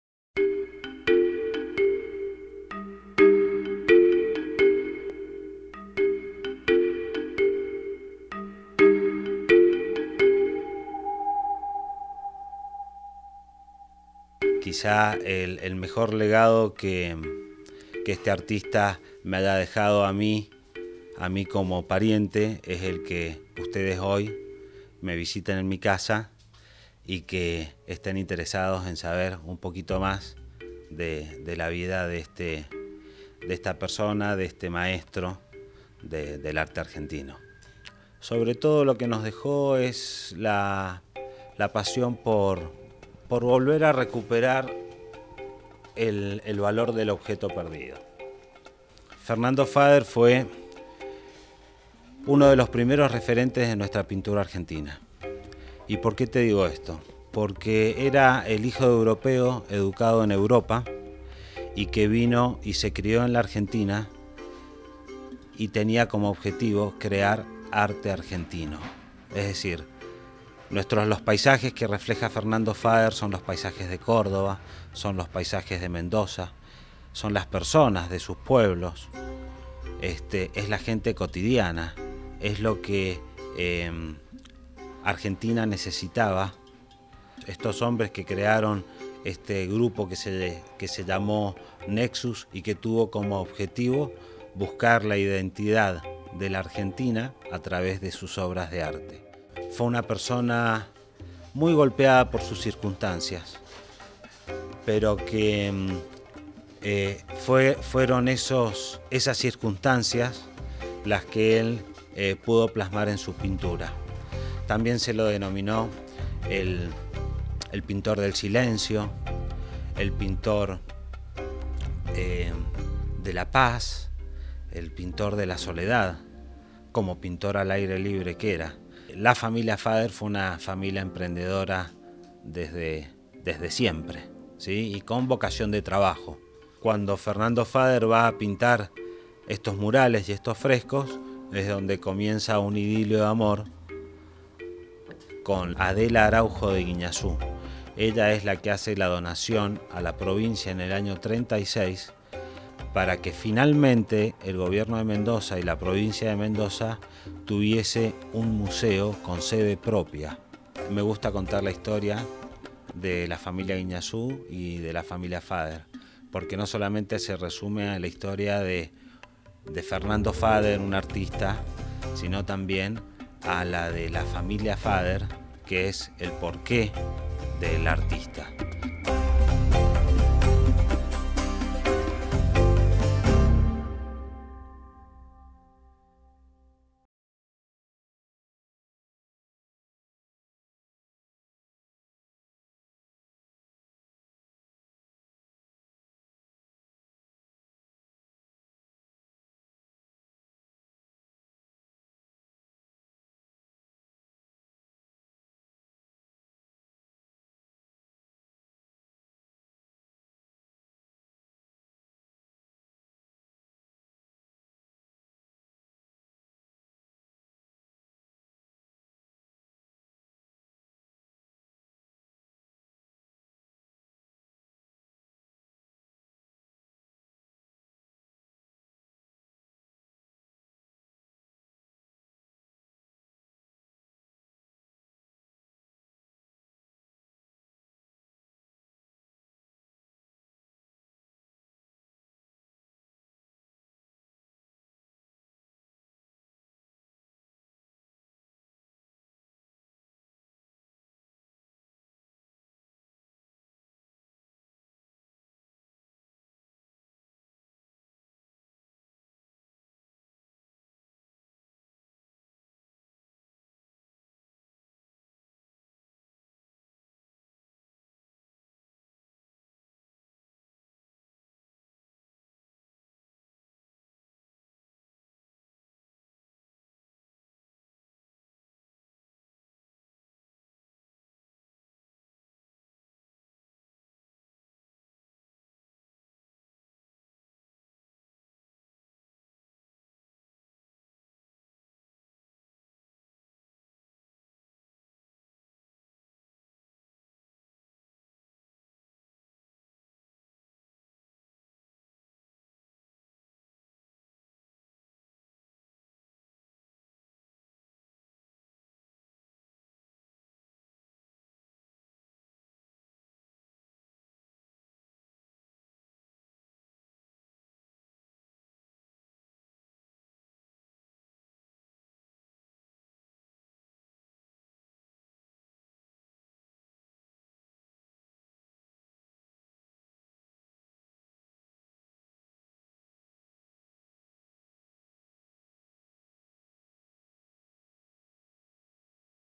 Homenaje a Fernando Fader
A 132 años del nacimiento de Fernando Fader, su bisnieto Lisandro Guiñazú, remomoró la histroria de su falimia y habló sobre el pintor que se radicó en Mendoza.